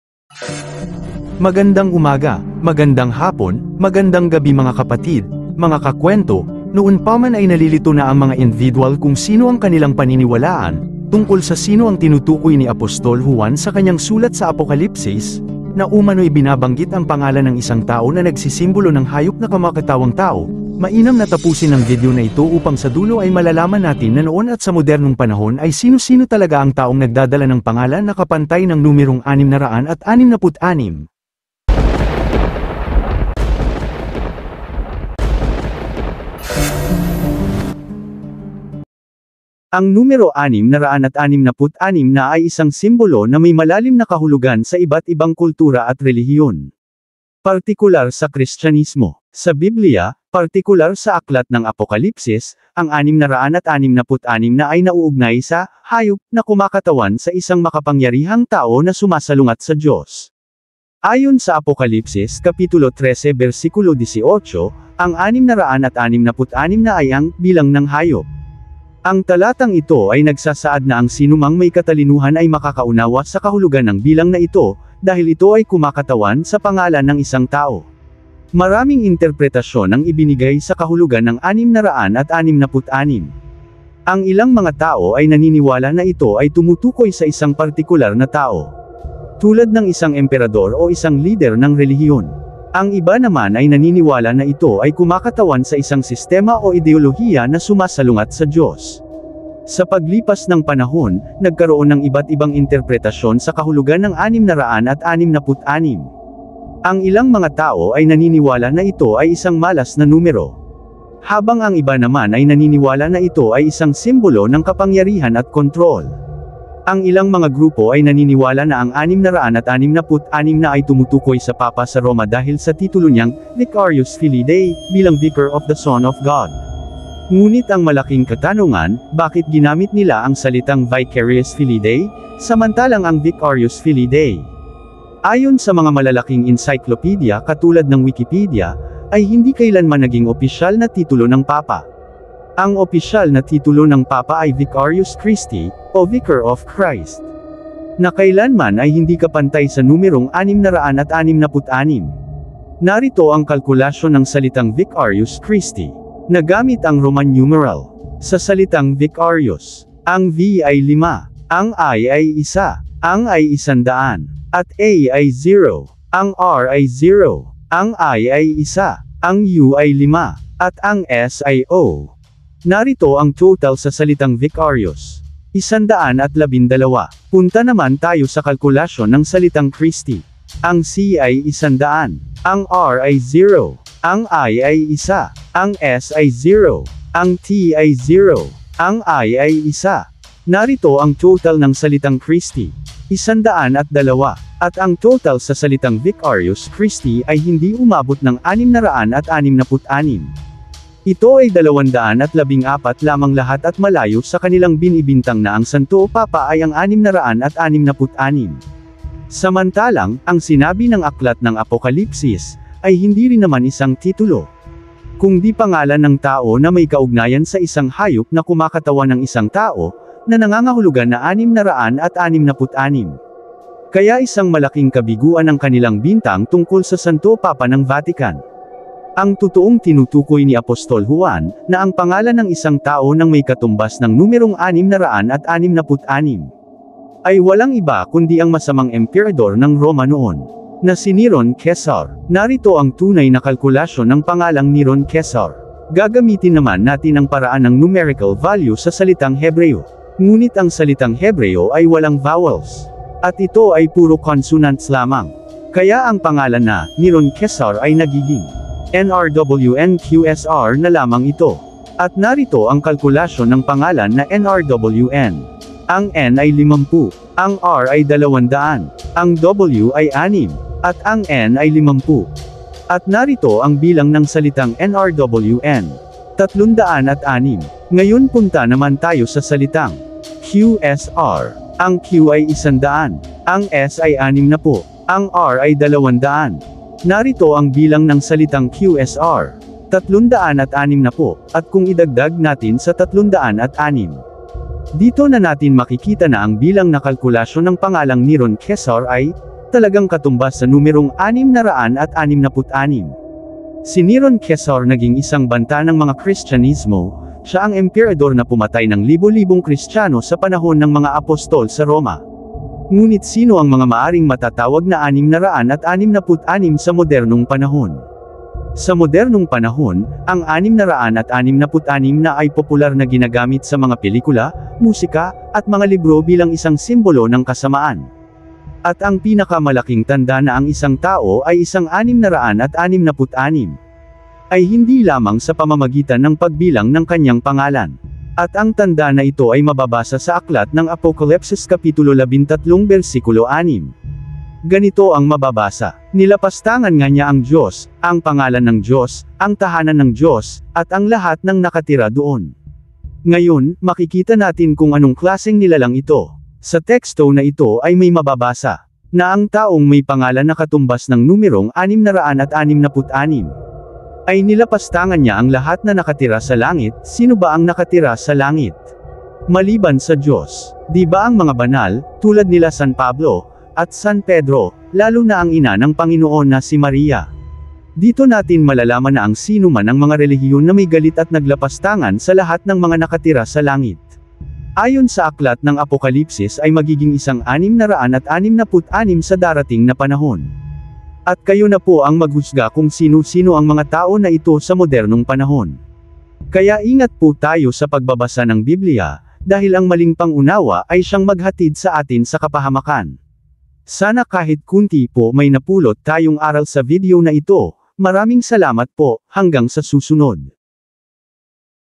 PAANO MAKILALA ANG TAONG 666 SA MODERNONG PANAHON
paumanhin po. ang video na ito ay ginawa hindi para mananakot ng iba at mag husga.

ito ay bahagi lamang sa isang propesiya ng biblia kung sino ang tunay na nag mamay ari ng numerong 666 ayon sa biblia.

at ito ay isa lamang babala galing sa biblia upang malaman natin ang totong kalagayan sa mga taong kaaway ng Diyos.